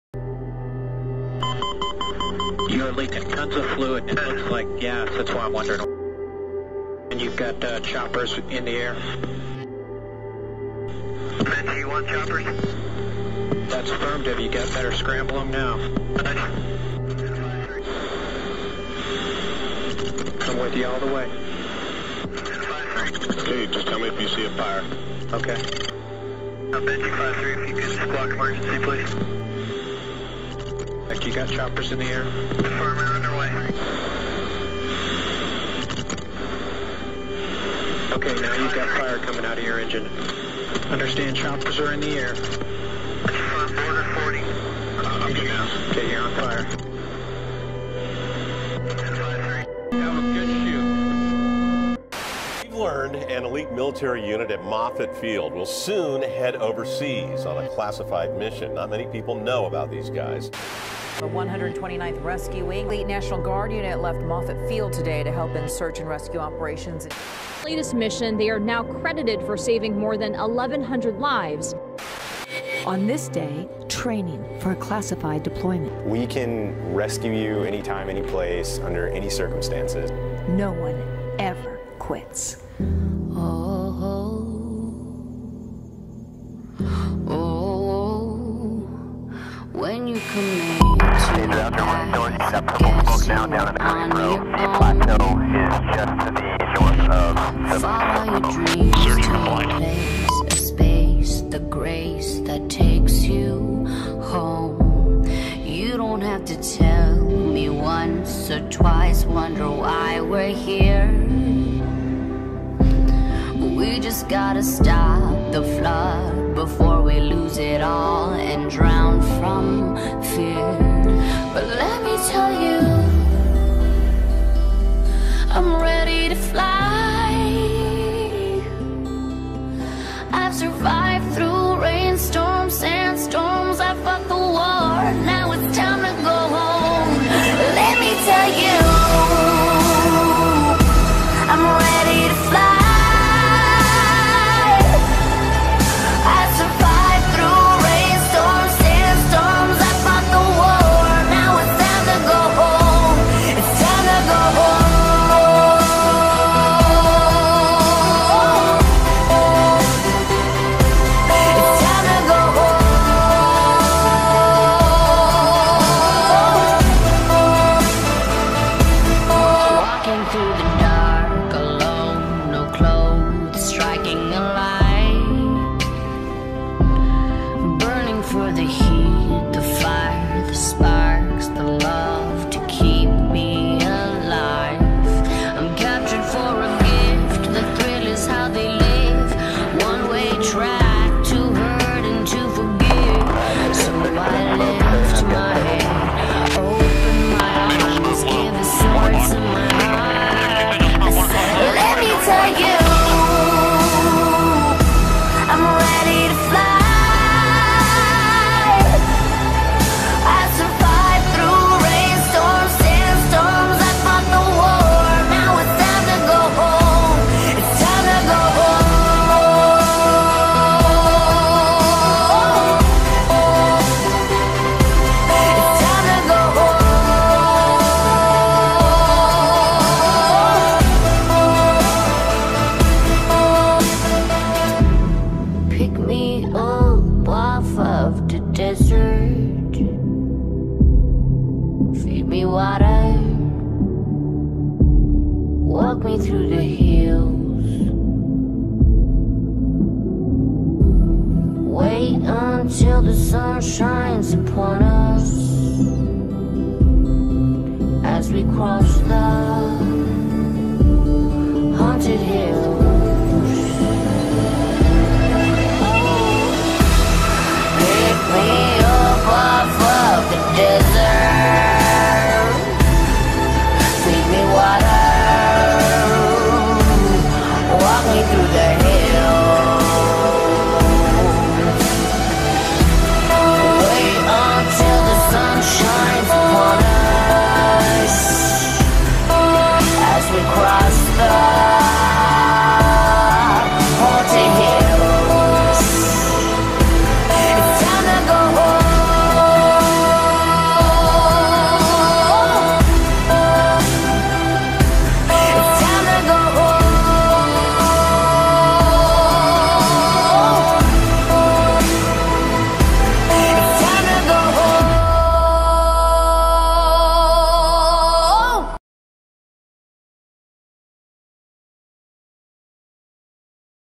COMBAT SEARCH AND RESCUE
Around the world special operations units rely on the extreme rescue and trauma skills of specialist military search and rescue units.

The need have all the skills of a paramedic, a mountain rescue team and be Abel to all of that behind enemy lines and under fire.